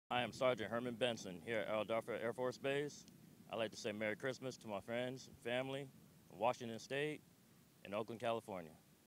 Sgt. Herman Benson (2019) 🇺🇸
🔴 [Author]:
Video by Tech. Sgt. Joshua Williams

🔴 [Unit]:
380th Air Expeditionary Wing Public Affairs

🔴 [Description]:
Sgt. Herman Benson sends a holiday shout out to family and friends.

🔴 [Tags]:
holiday season

🔴 [VIRIN]:
VIRIN_NOT_FOUND